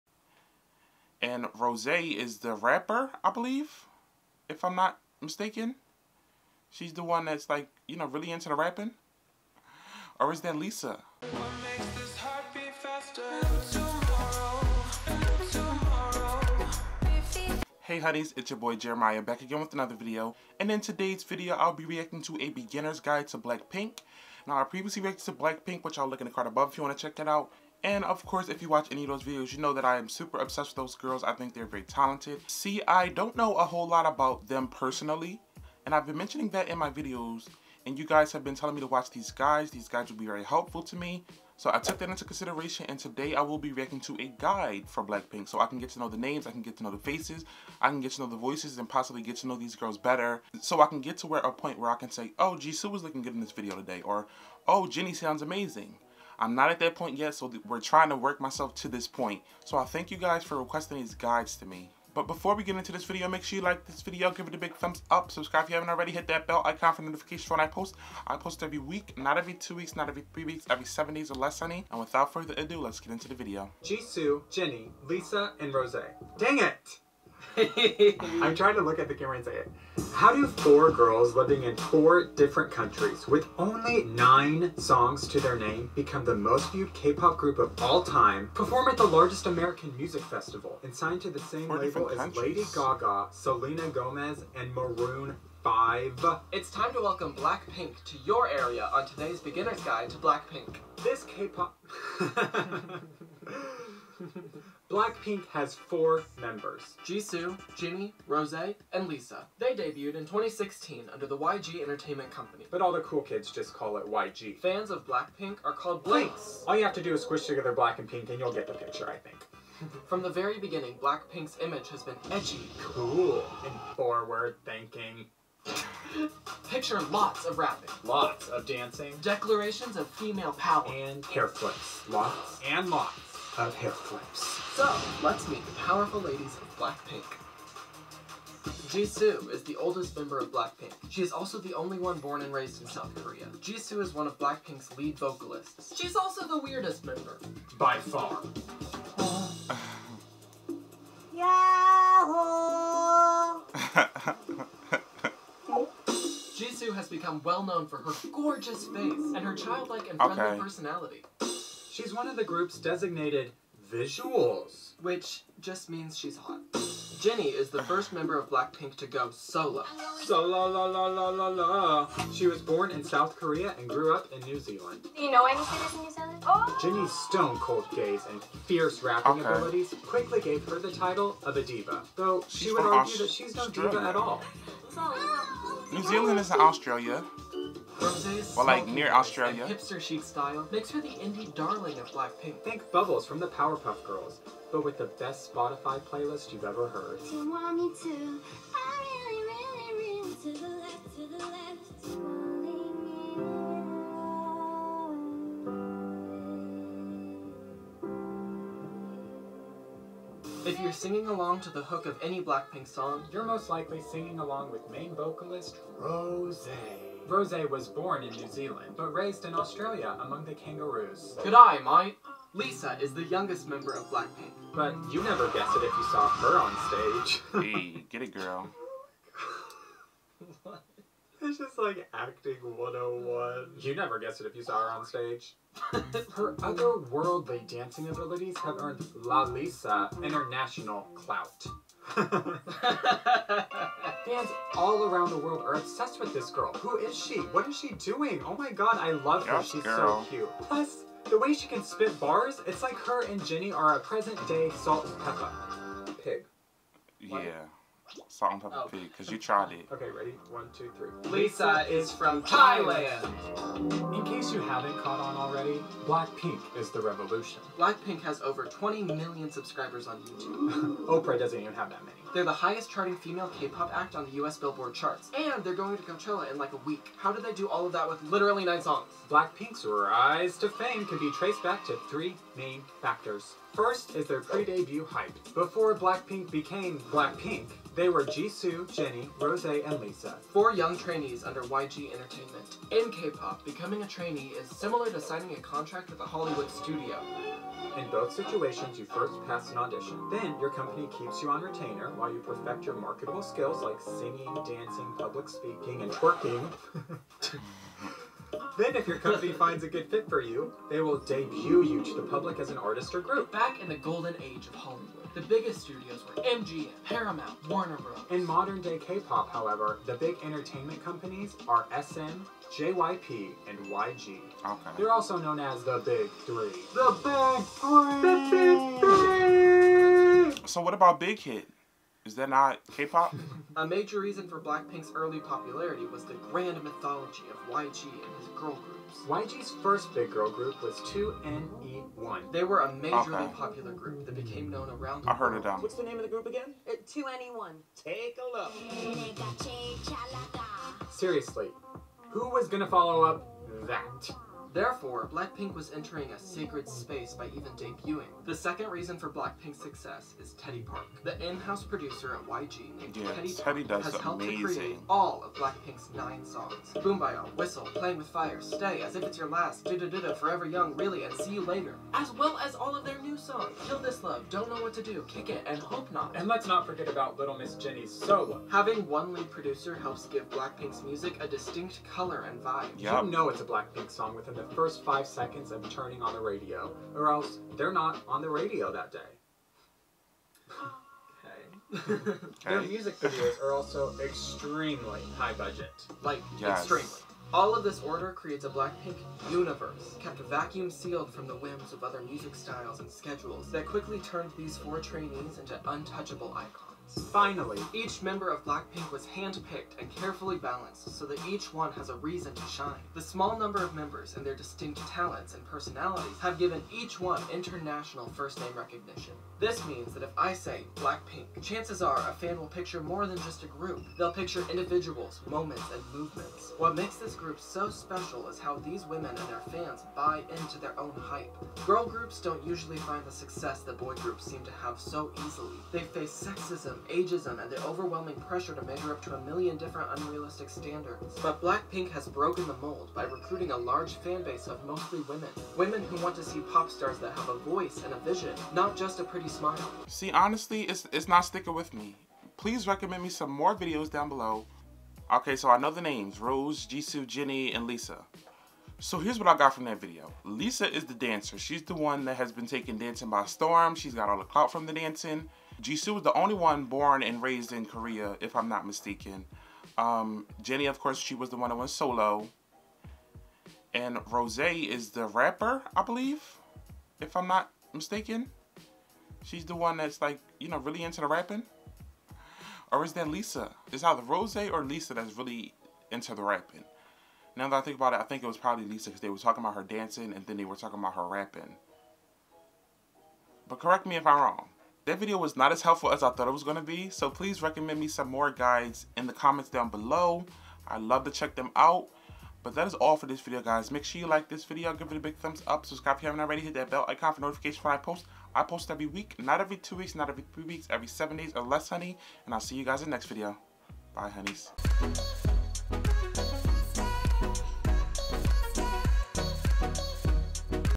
A BEGINNER'S GUIDE TO BLACKPINK (REACTION)
In this video I react to a beginners guide to BlackPink by Gay Pop.

Not every two weeks, not every three weeks, every 7 days or less honey!